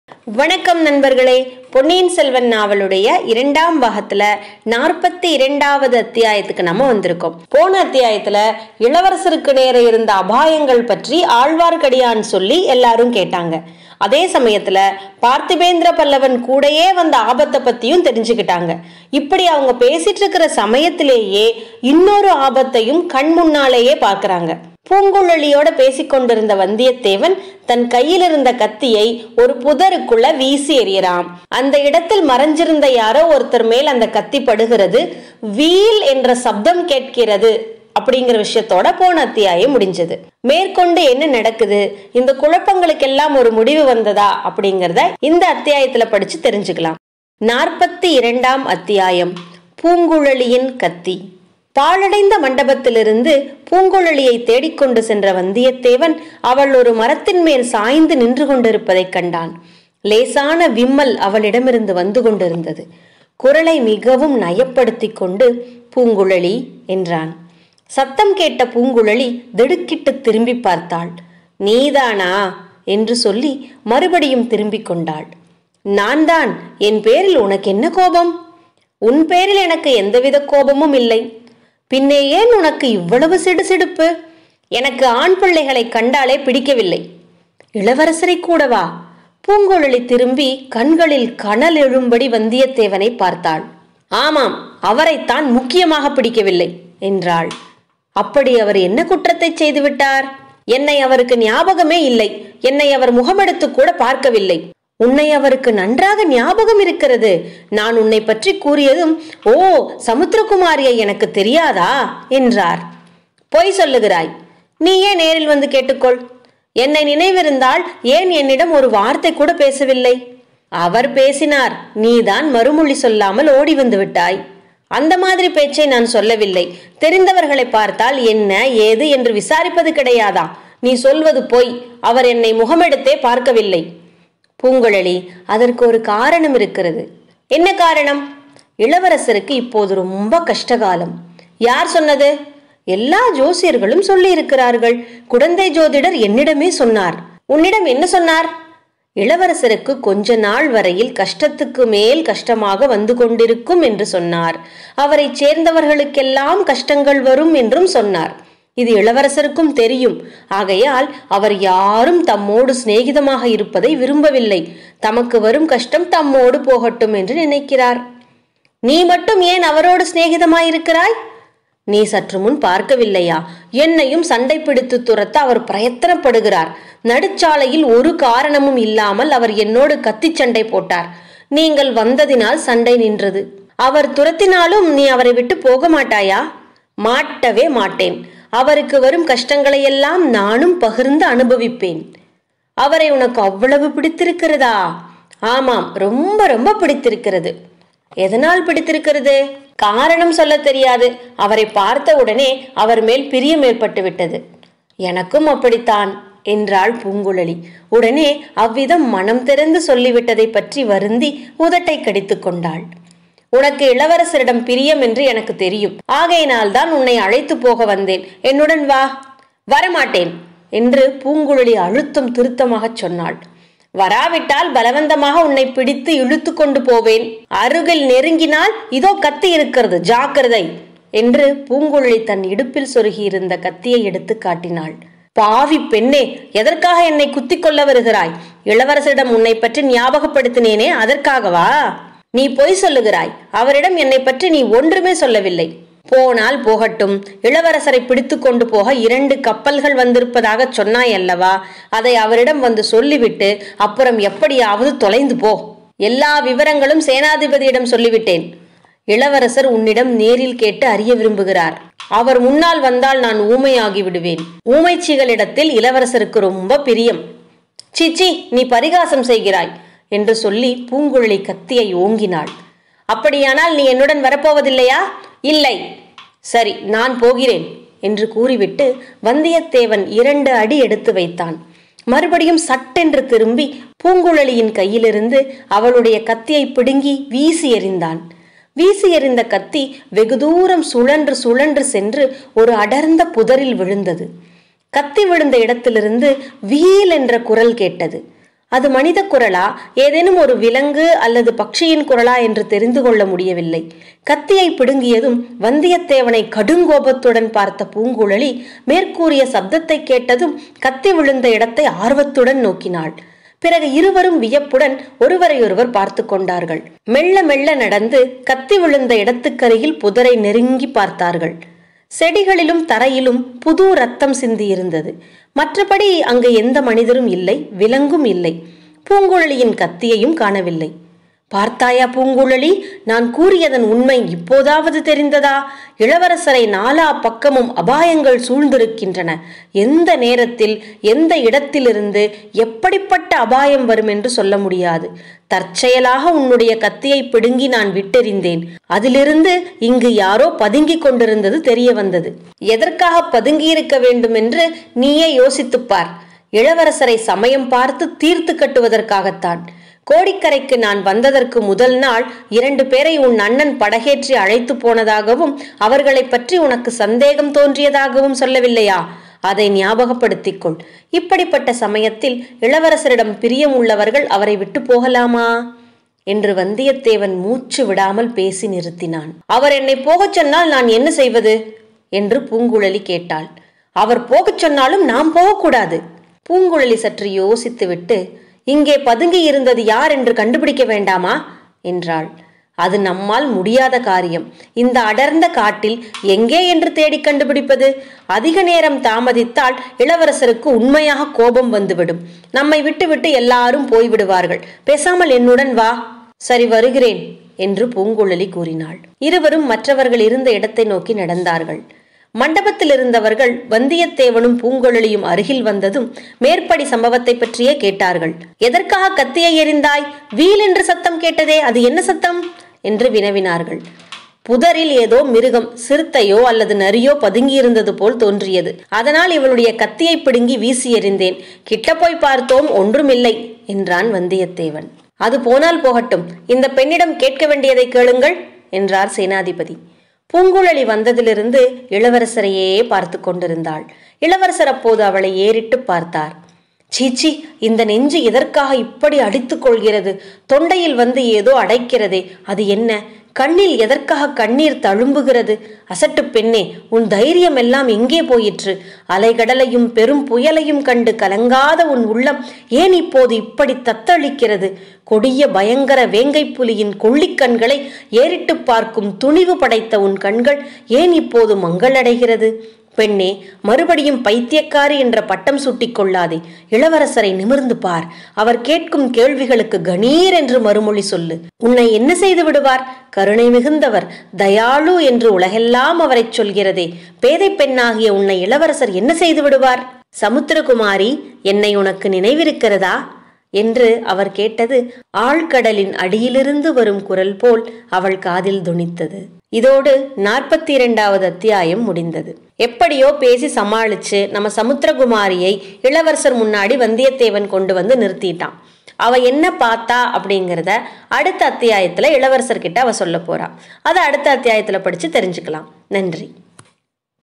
பொன்னியின் செல்வன் பாகம் 2 அத்தியாயம் 42 பூங்குழலியின் கத்தி
பூங்குழலியை சமாதானப்படுத்தினான் வந்தியத்தேவன்.